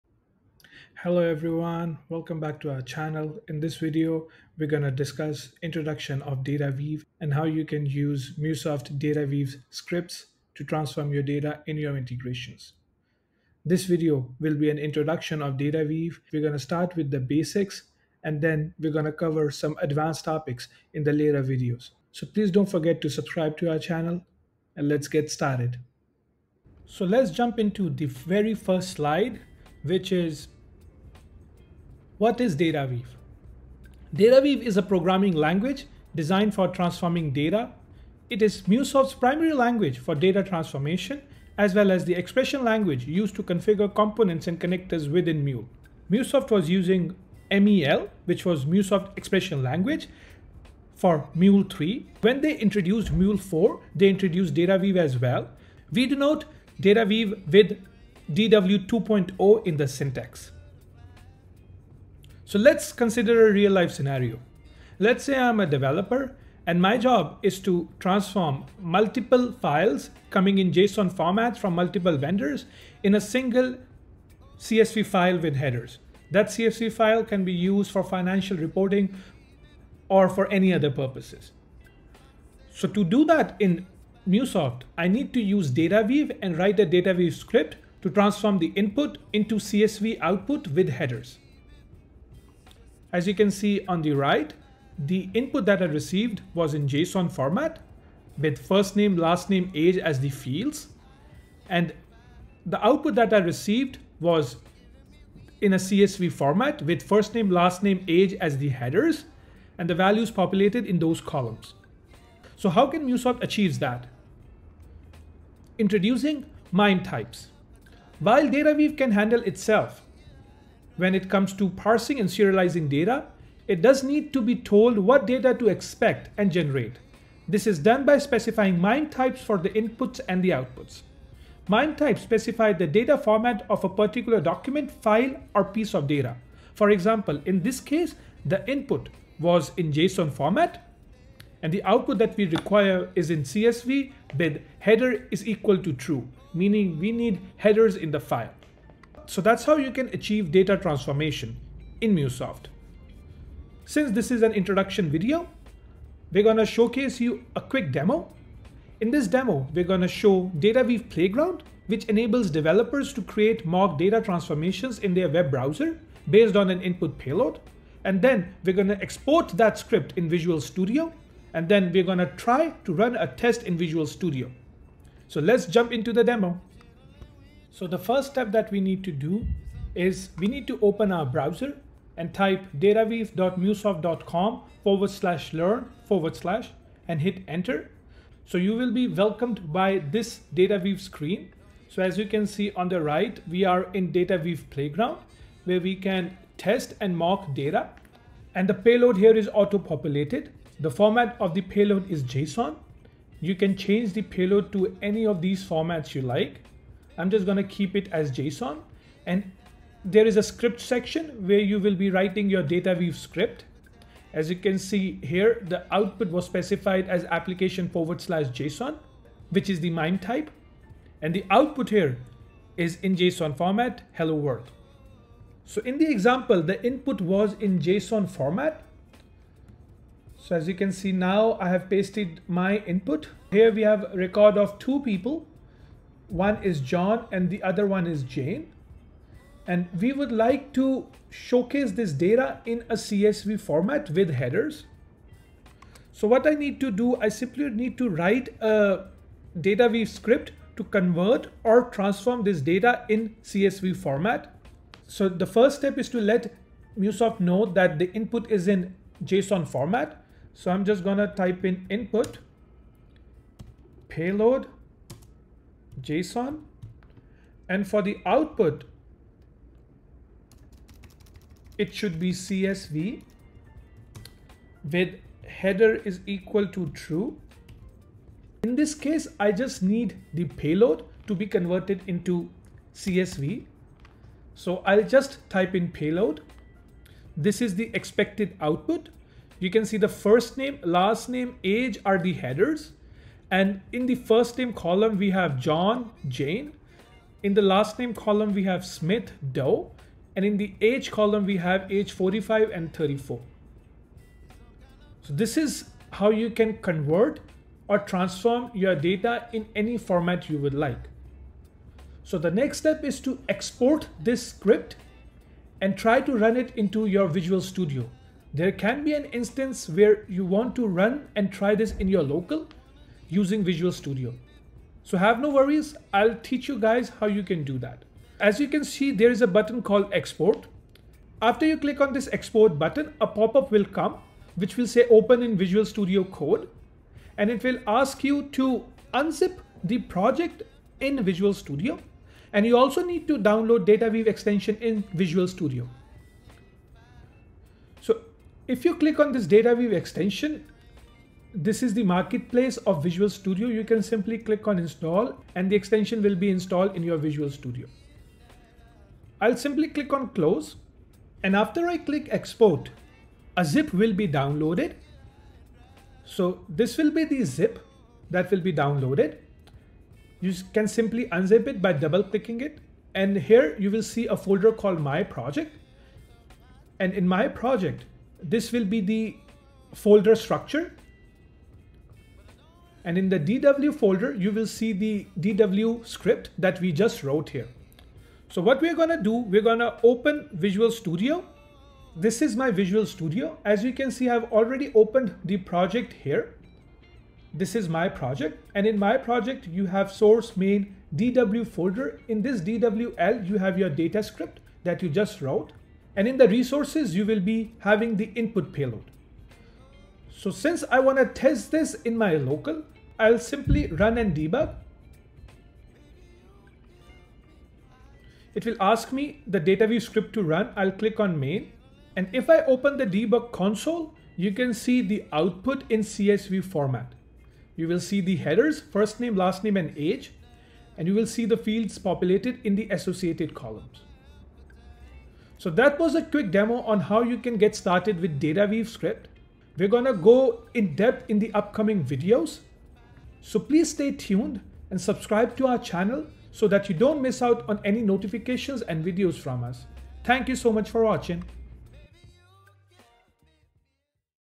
Introduction to MuleSoft DataWeave Series: Getting Started with DataWeave | Part-1
Hello Folks,

Join us in this exciting new tutorial series on MuleSoft's powerful transformation language, DataWeave. In this introductory video, we dive into the fundamentals of DataWeave and explore its capabilities for data transformation and mapping.

If you're new to MuleSoft or looking to enhance your integration skills, this series is perfect for you. In this first video, we'll cover the basics of DataWeave, including syntax, playground, and exporting code to Visual Studio. You'll learn how to transform data from one format to another using DataWeave.

Throughout the series, betweenTwomules will guide you through hands-on examples and provide useful tips and tricks to simplify your integration workflows. By the end of the series, you'll have a solid foundation in DataWeave and be able to confidently leverage its power to solve complex data transformation challenges.

Don't miss out on this opportunity to expand your MuleSoft mulesoft skills and improve your integration projects.

- timestamps-
00:00 - Intro!
00:30 - Intro Dataweave
01:04 - Example!
01:50 - Introducing MIME Type!
02:32 - Demo
07:15 - Closing Statements

2. Dataweave Extension for VS